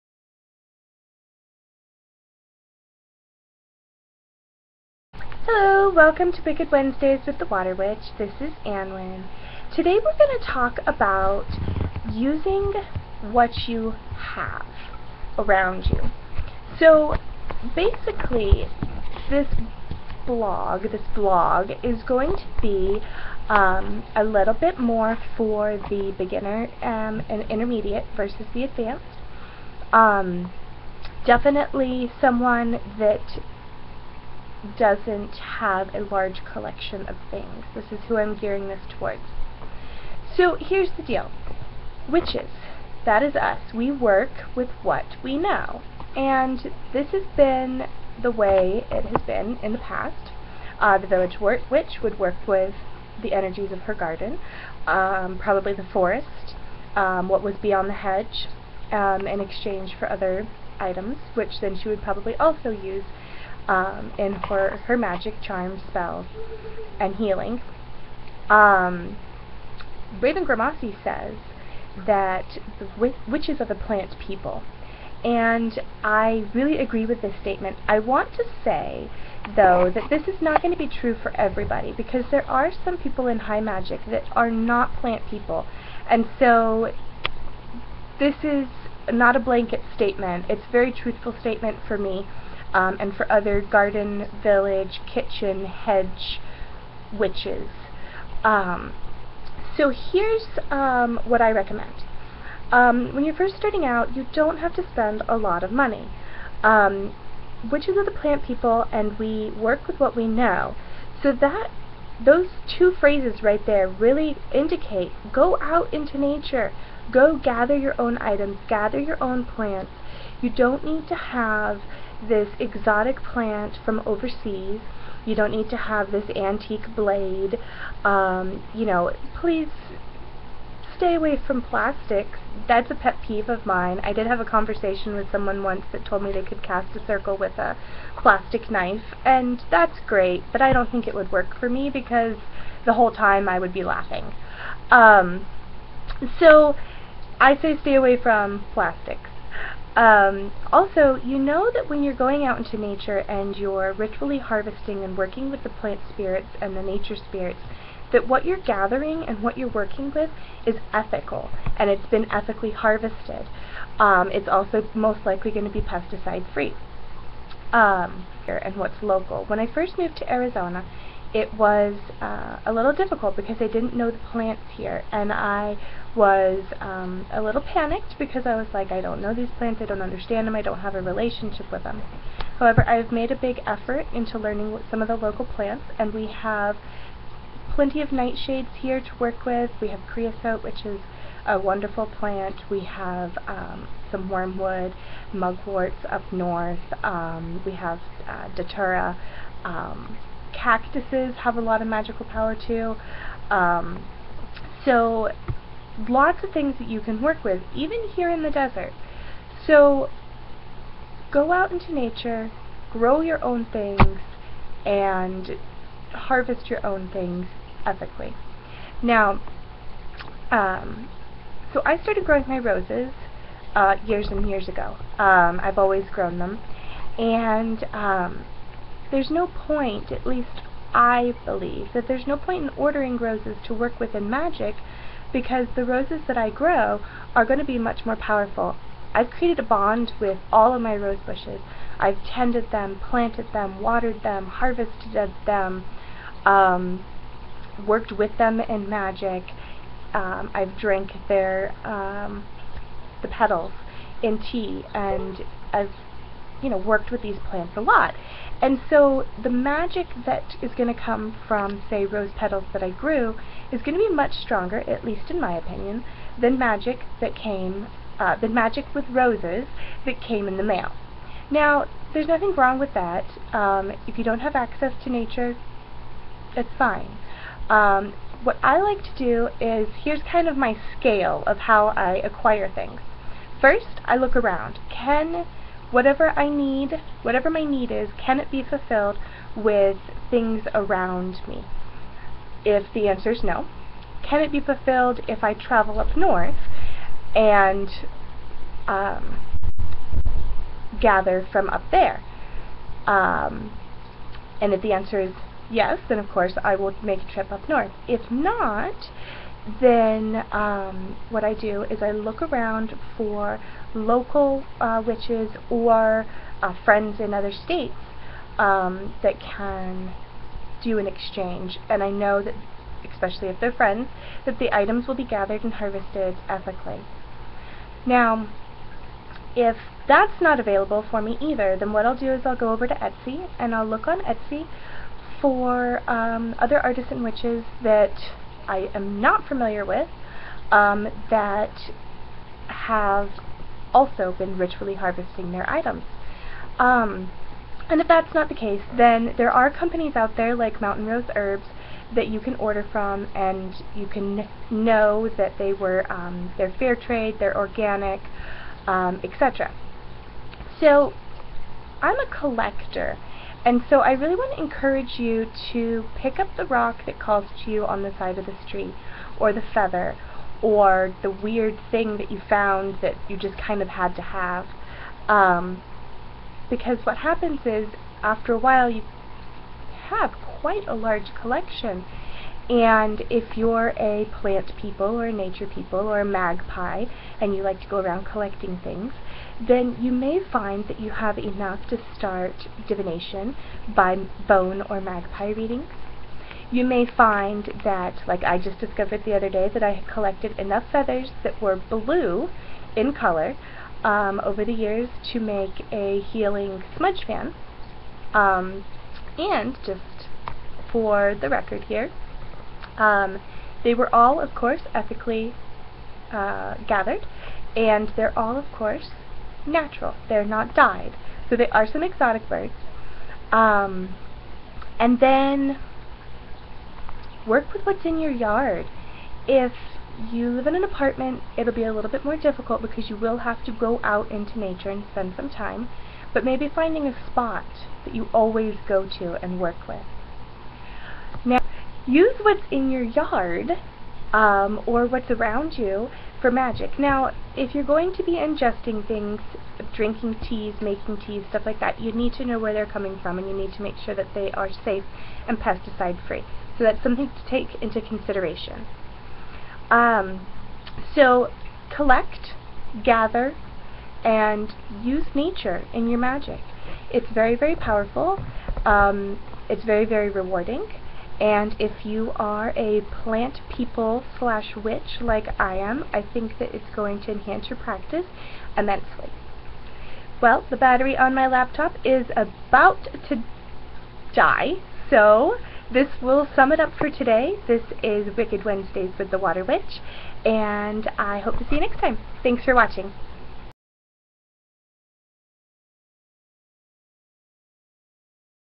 Using nature around you for magic- Water Witch
Where I ramble about using nature and the things around you rather than plastic knives and store bought things.